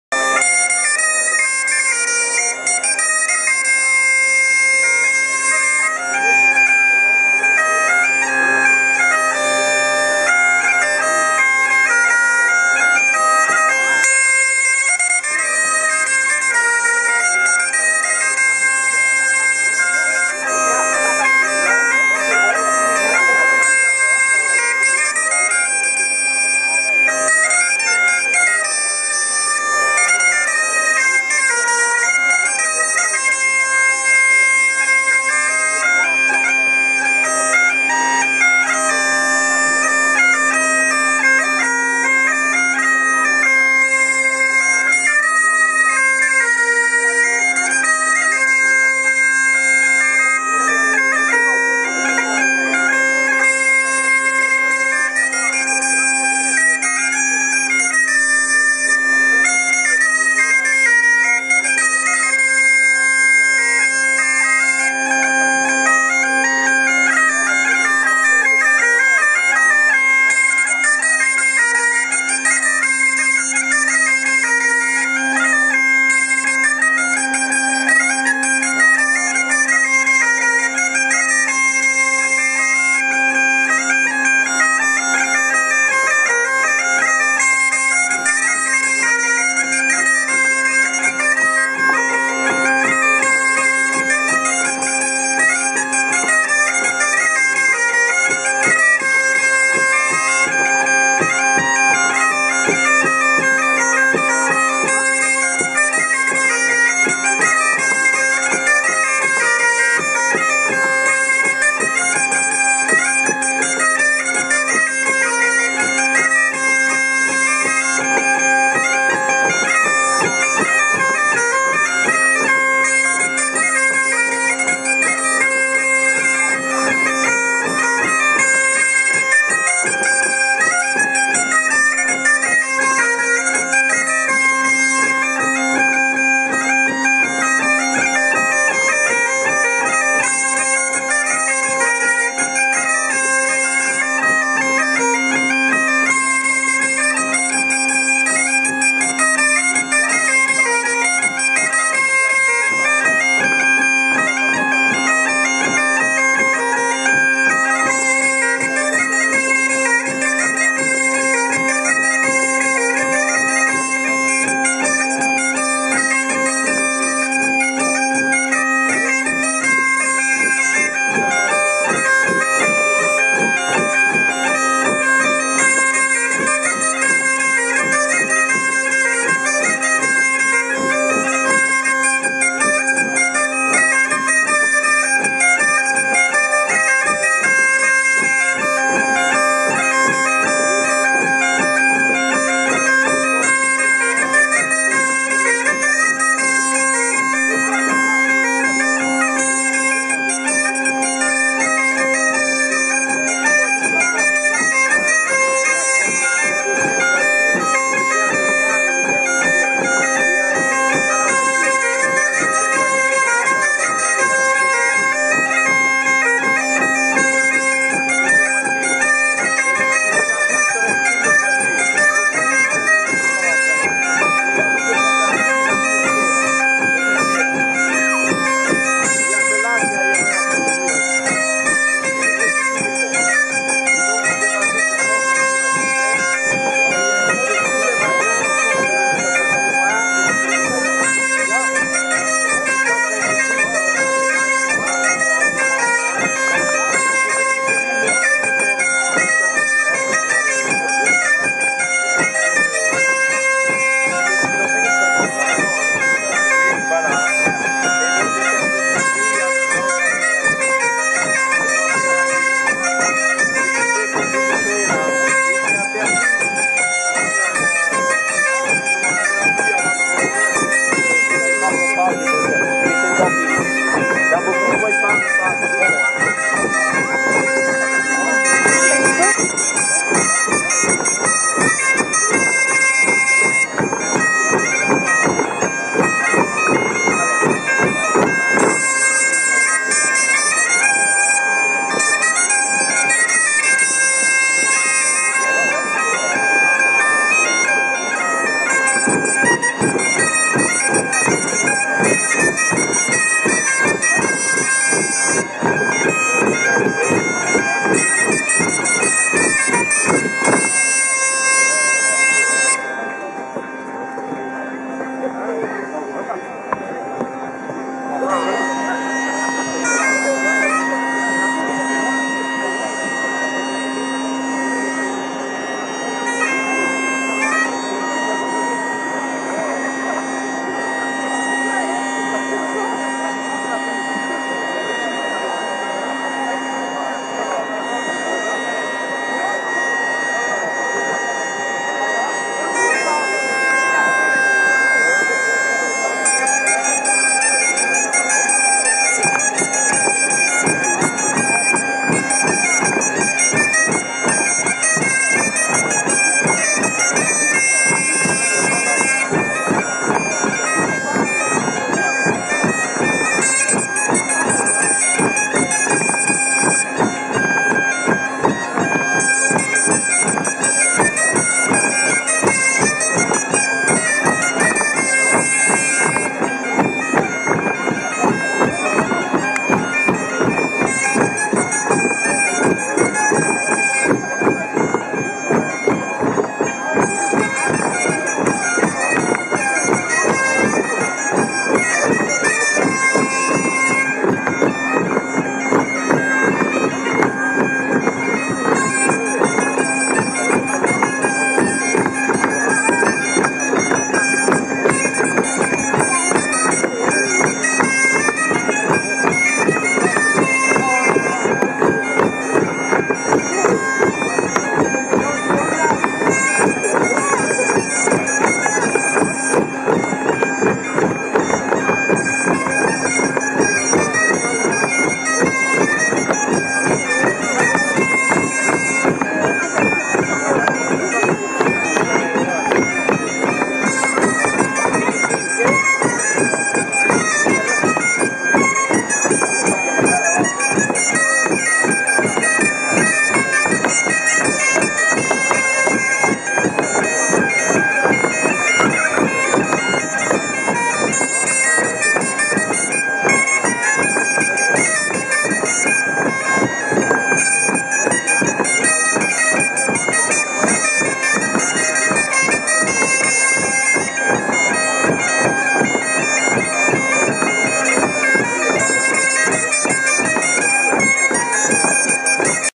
Festival Plinn du DANOUET - concours sonneurs braz
Le lundi 15 août 2011, au Danouët près de Bourbriac. Les sonneurs pourraient être Le Moal/ Le Berre.